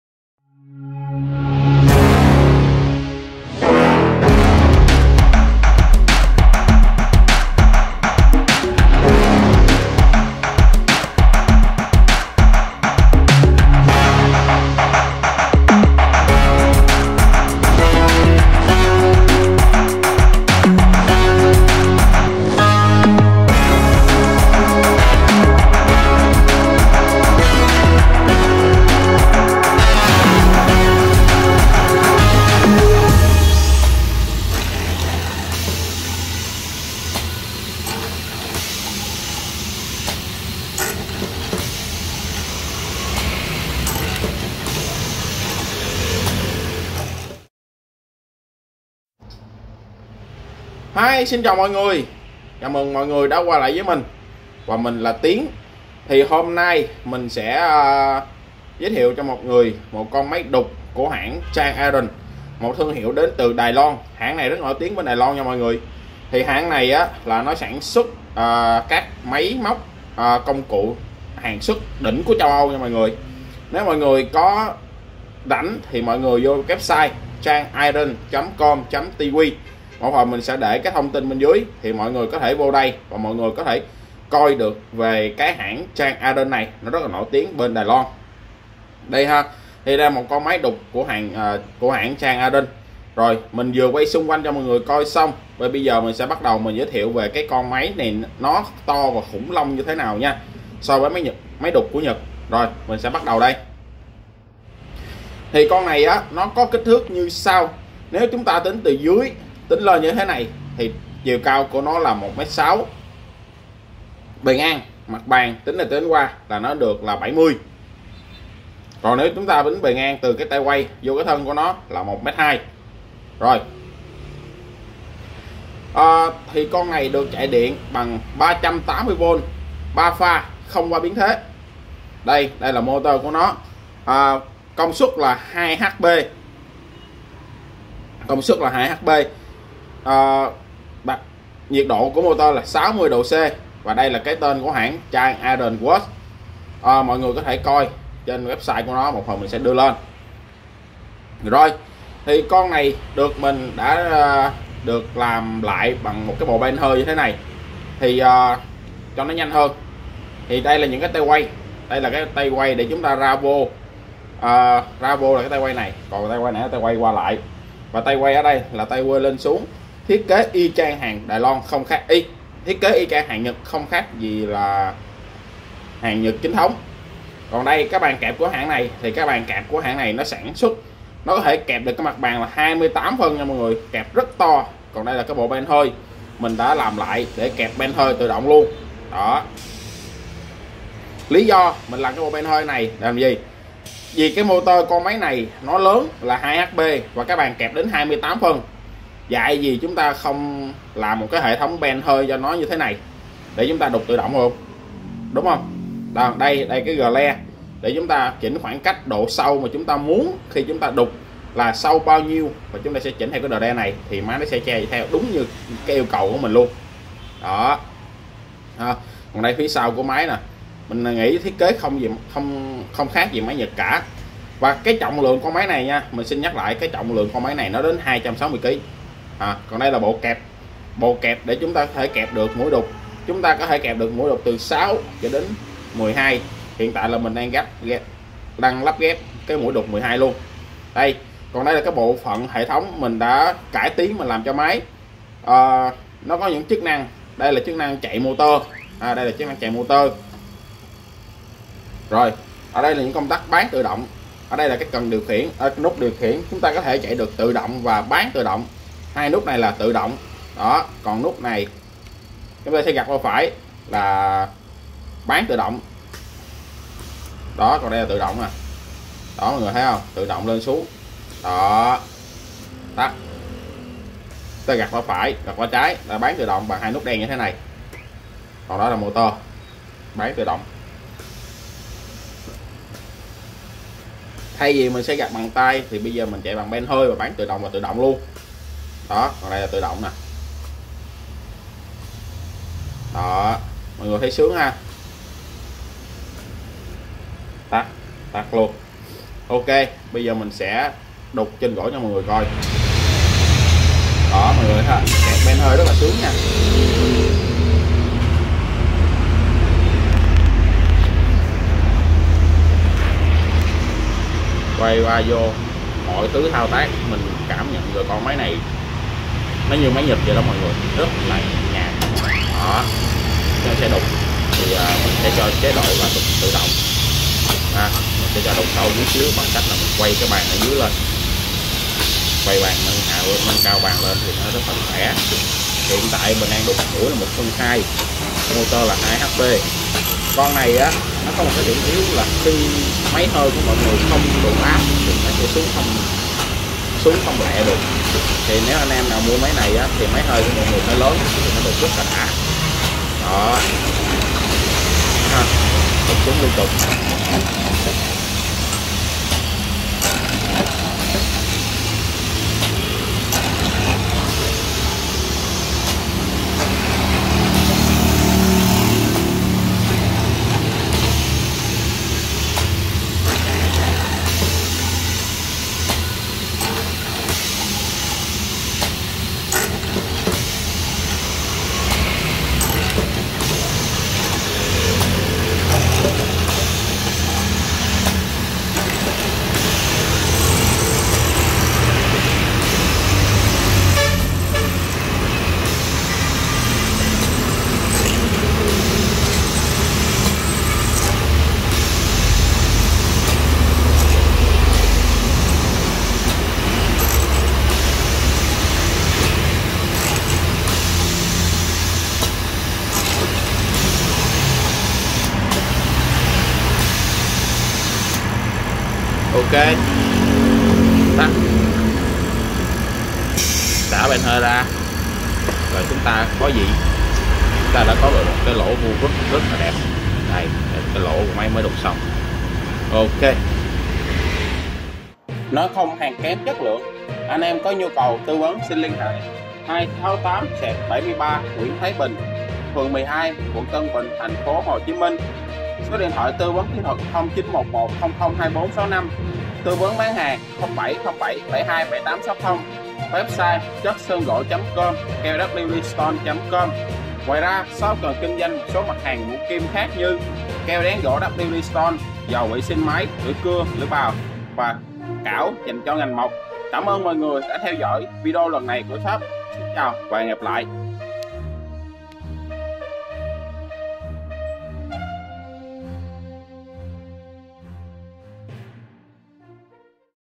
Máy đục mộng vuông Chang Iron 02 / Máy đục mộng vuông đài loan / Máy đục chế biến gỗ đài loan
👍👍" Nói KHÔNG hàng kém Chất Lượng " 👍👍
Kích thước : 1200*7000*1600 mm
Trọng lượng : 260 kg
Điện : 380v 3 pha / Moto 2 hp
Bàn kẹp tối đa 28 cm
Mũi đục từ 6-12 ly
Máy được Shop chatsongo cải tiến hệ thống Pittong đục tự động và bán tự động theo nhu cầu người sử dụng
Hệ thống kẹp ben hơi vững chắc thao tác nhanh hơn và chắc chắn hơn
💰💰Giá Tham Khảo : 2x.000.000 ( triệu )
💥💥Thay đổi không đau đớn Chống lại sự thay đổi mới đau đớn 💥💥
📍268/73 Nguyễn Thái Bình P12 Quận Tân Bình TpHCM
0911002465 ( Tư vấn kỹ thuật )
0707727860 ( Tư vấn bán hàng )